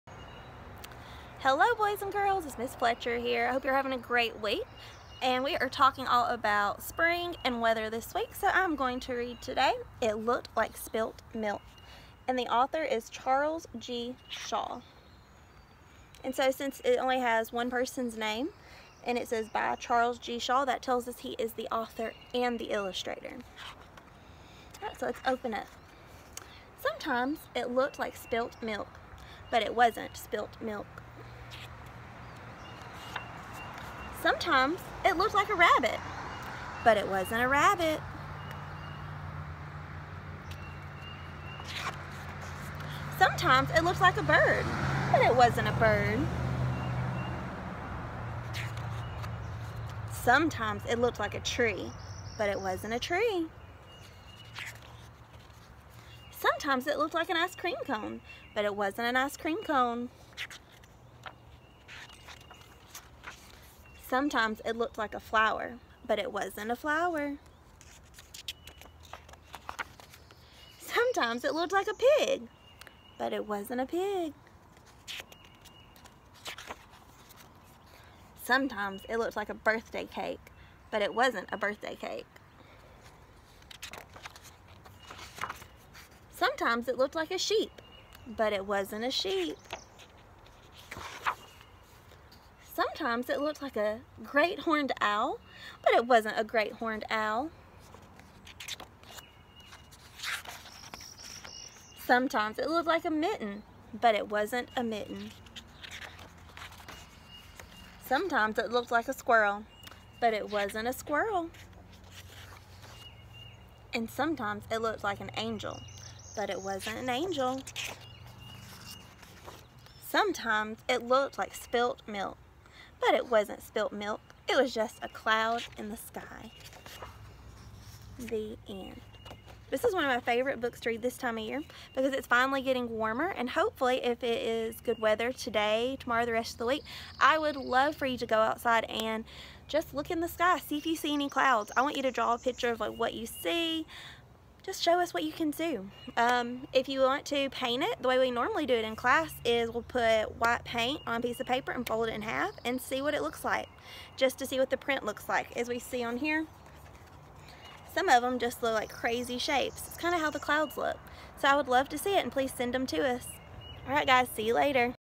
It Looked Like Spilt Milk ( read by Miss. Fletcher)
Hello boys and girls, today we are reading “It looked Like Spilt Milk.” I would love for you to go outside and look at the clouds afterwards, see what shapes you can see!